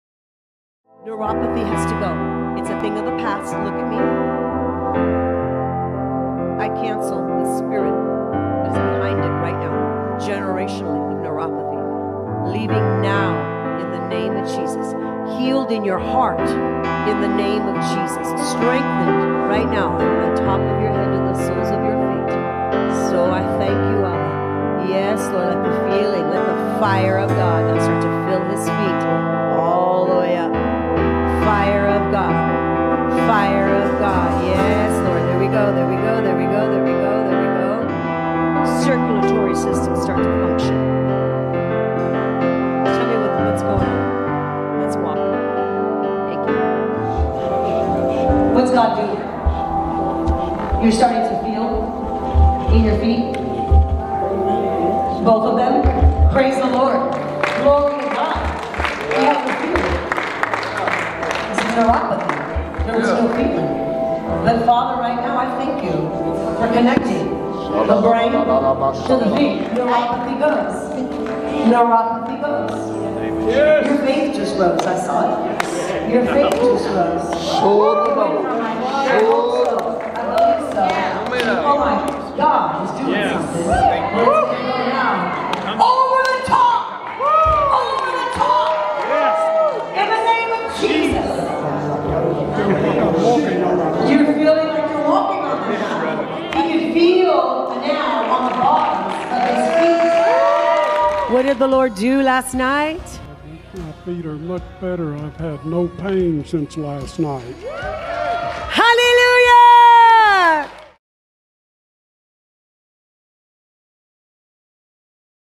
Healings - Neuropathy in the Feet
While receiving prayer for Neuropathy in his feet, he could start to feel the ground!  He came back to testify the next night during the 3 Night Limitless Mississippi Crusade that there was no pain in his feet!  Incredible move of God’s Spirit!
Mark 9:23
Jesus said to him, “If you can believe, all things are possible to him who believes.”